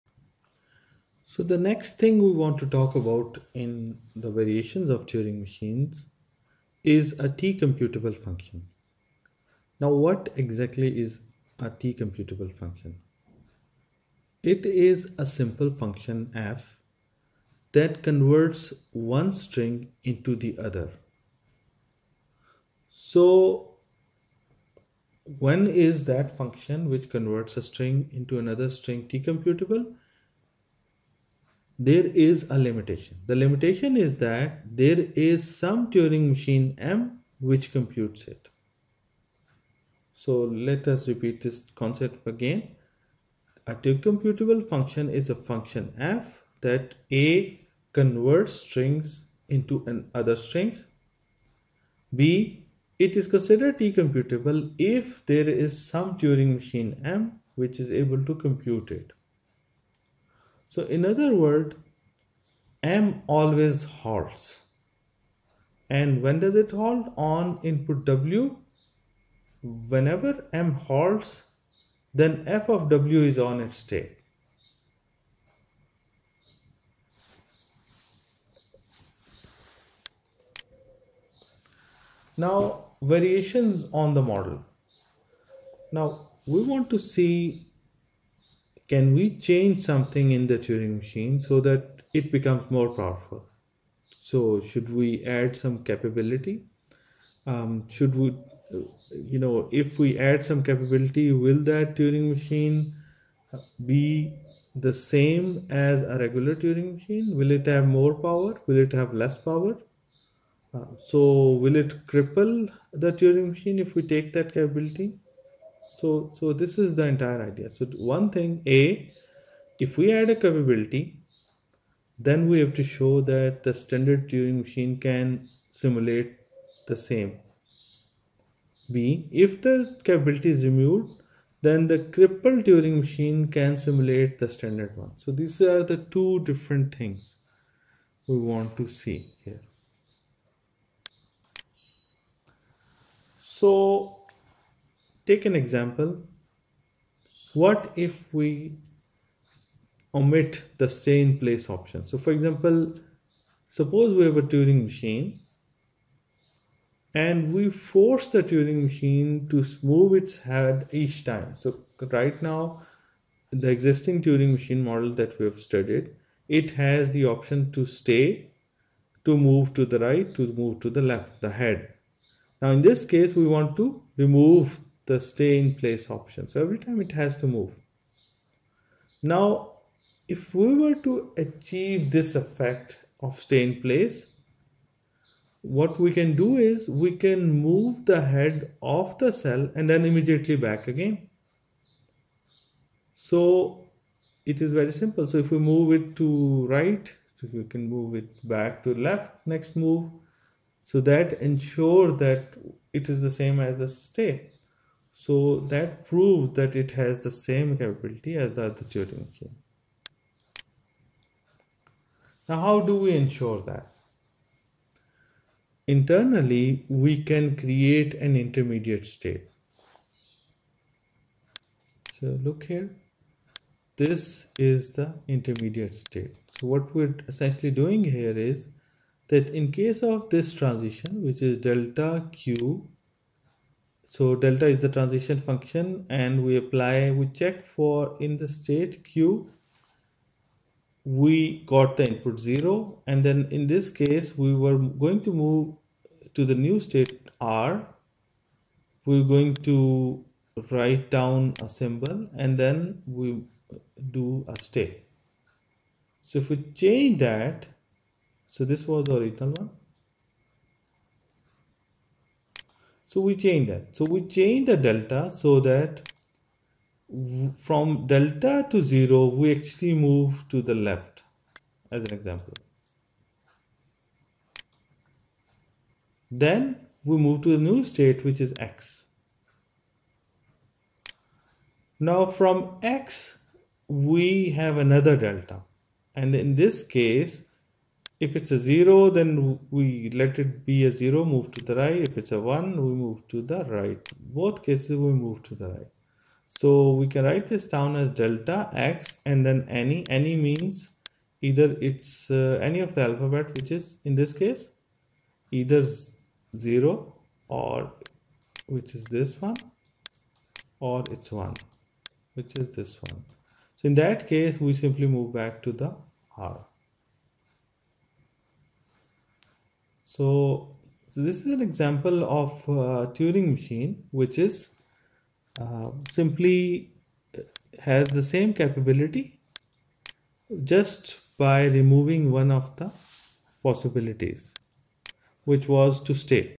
Turing Machine Variations (T-Computable functions)
Here we discuss Turing Machines Variations - T Computable Functions and Turing Machines which omit stay-in-place.

Photo by Magda Ehlers from Pexels

Slides credit Prof. Wayne Goddard, Book author